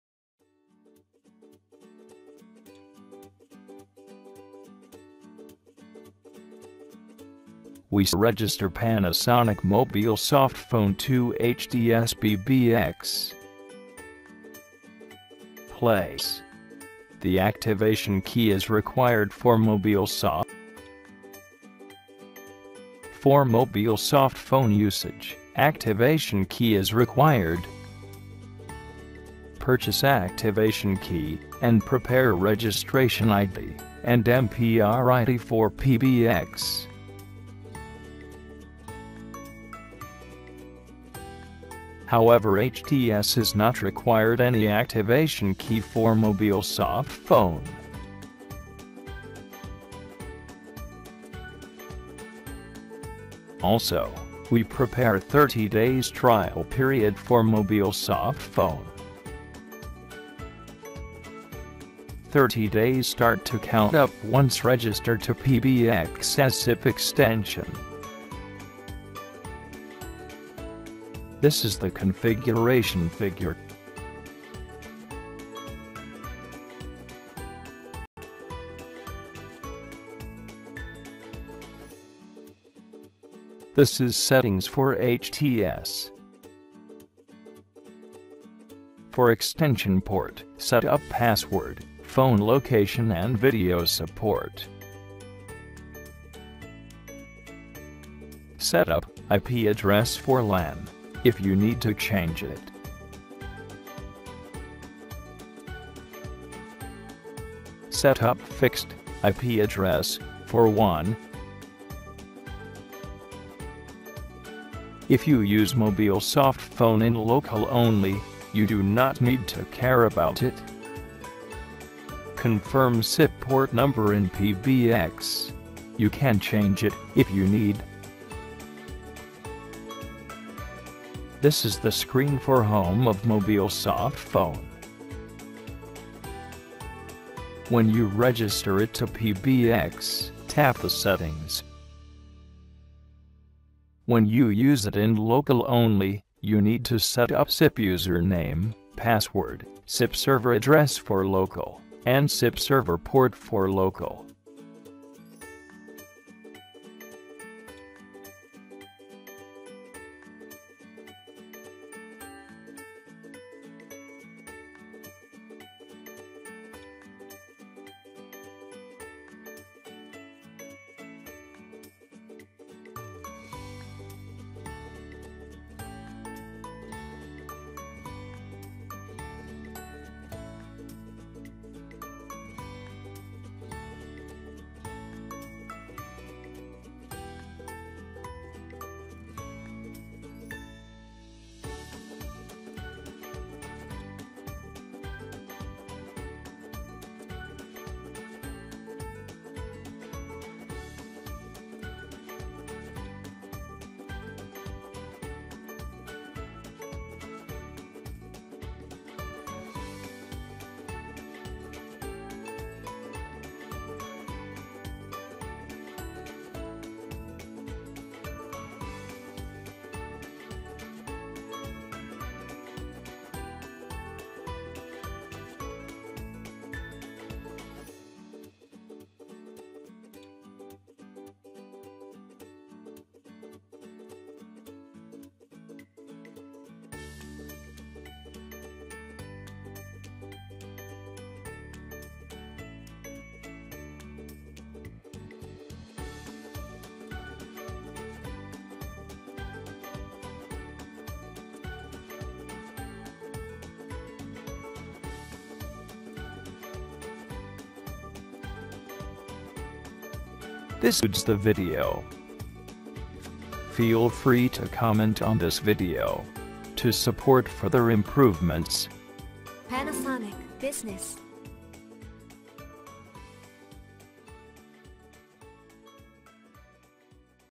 Como dar de alta y registrar en un Conmutador Panasonic KX-HTS - Panasonic Softphone Móvil
Panasonic Softphone Móvil
Como dar de alta y registrar en un Conmutador Panasonic KX-HTS

Aquí encontrara el procedimiento para dar de alta y registrar un Smartphone con el Programa Panasonic Softphone Móvil instalado como una extensión de un Conmutador Panasonic KX-HTS

Les recordamos que en este equipo no se requiere llave clave de activación AK en el Conmutador Panasonic KX-HTS pero si se requiere comprar la licencia del Programa Panasonic Softphone Móvil KX-UCMA001W, KX-UCMA005W, KX-UCMA010W, KX-UCMA025W o KX-UCMA050W

Para comprar y más información sobre el computador Panasonic KX-HTS puede visitar la pagina

Para comprar la licencia y más información puede visitar la pagina